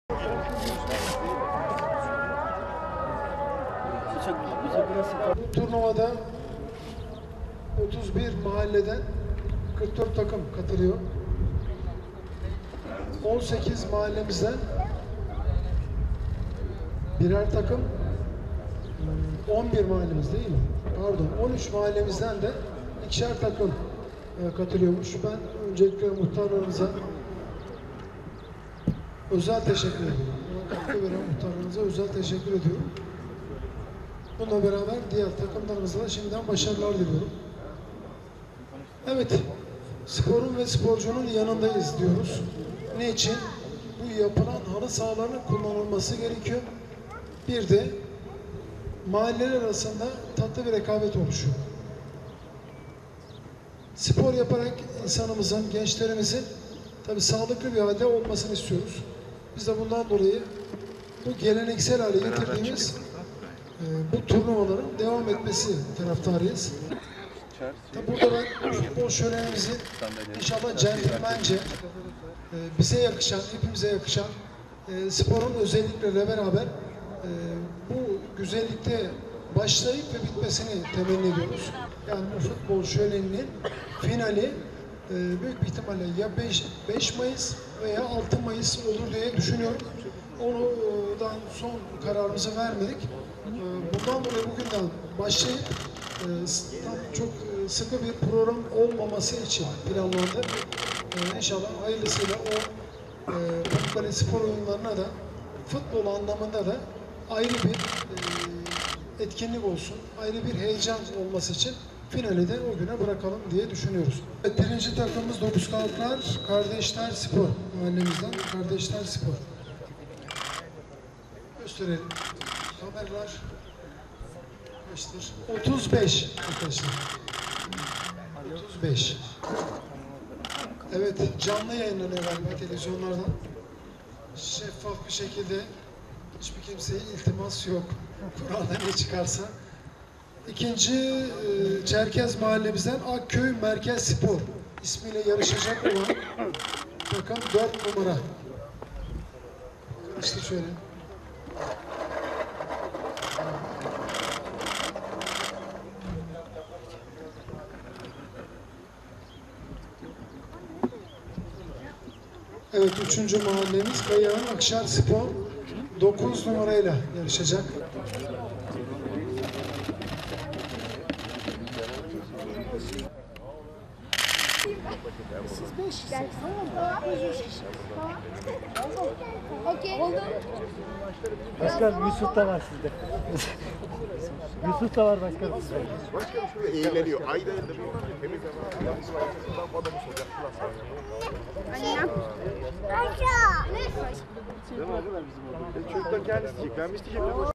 Eski bir gelenek ‘mahalle maçları’ Pamukkale’de devam ediyor
Pamukkale Belediyesi’nin geleneksel olarak düzenlediği organizasyonlardan olan ‘Pamukkale’de Futbol Şöleni’nin yedincisi ‘İlkbahar 2018’ adıyla yapılacak. Mahalleler arasında kaynaşmayı amaçlayan turnuvanın kura çekimi yapıldı